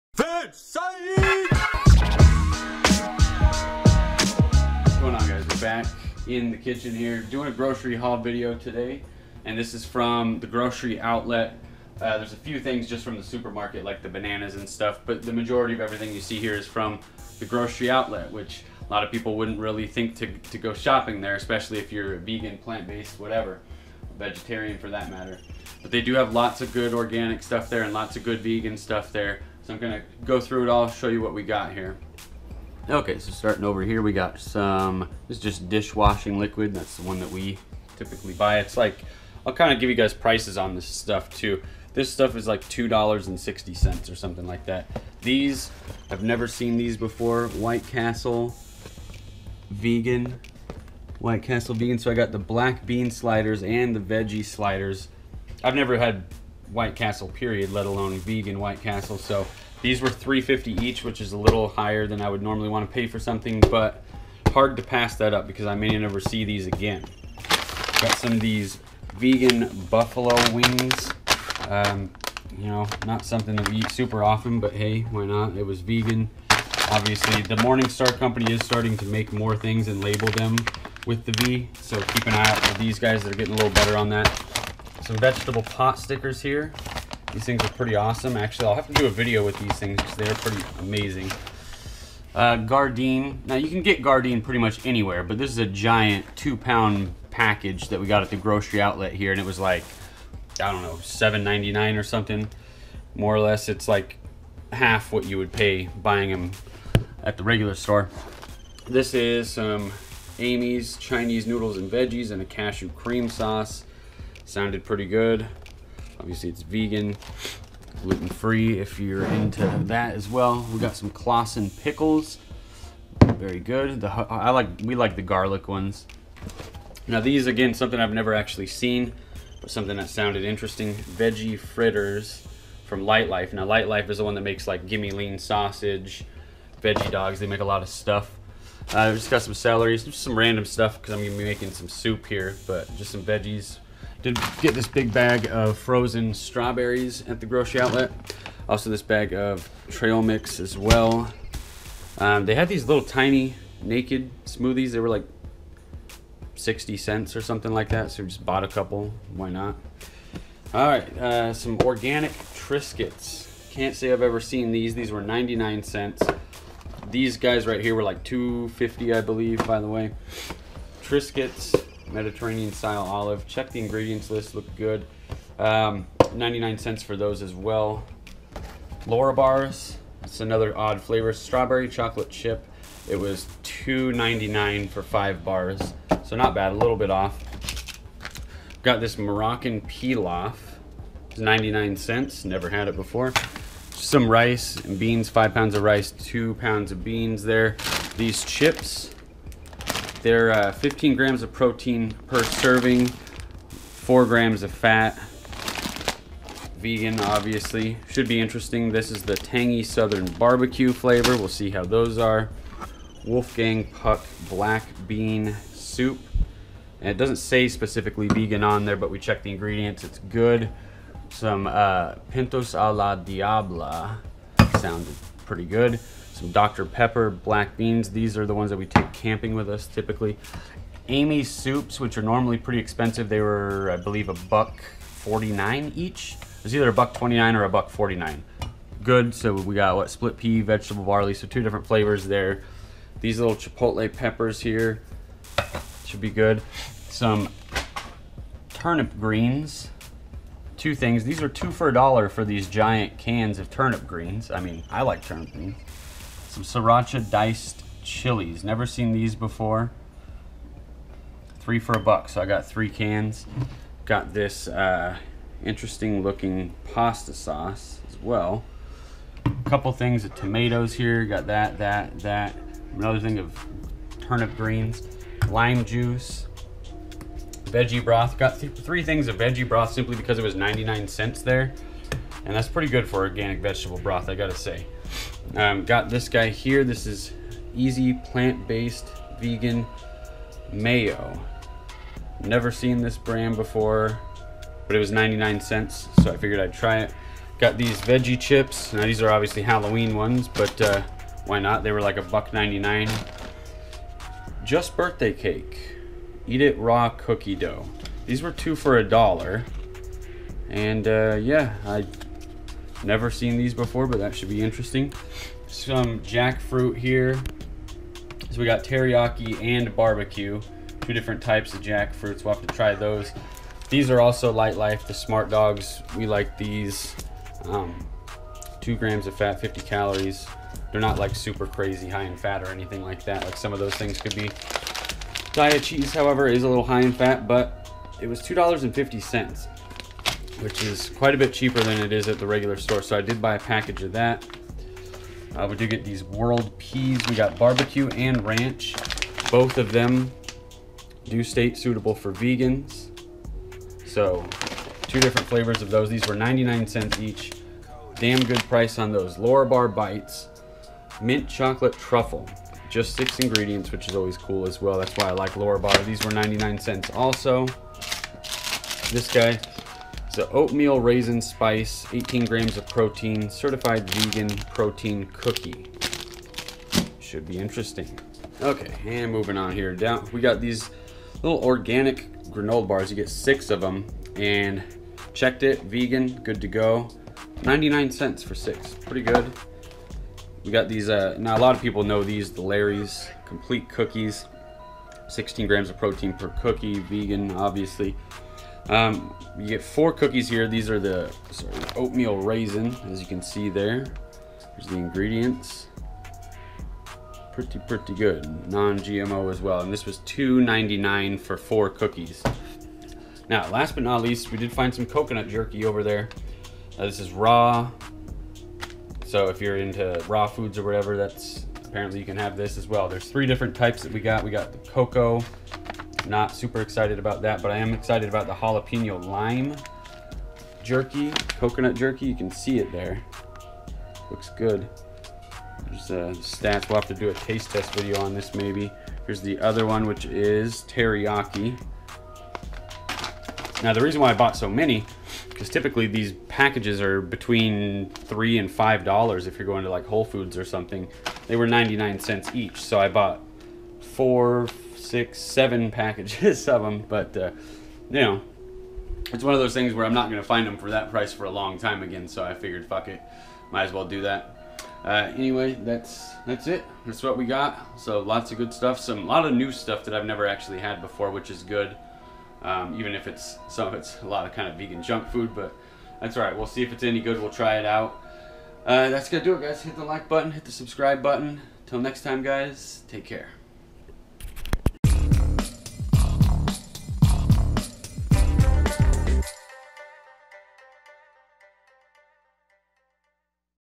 Grocery Outlet Madness - Vegan Food Haul
Guys we found all kinds of Vegan stuff at the Grocery Outlet. If you haven't gone shopping there for Vegan food items you are missing out. Lets see what we got!

If you see anything that you would like to see me use in a recipe video post it the comments down below.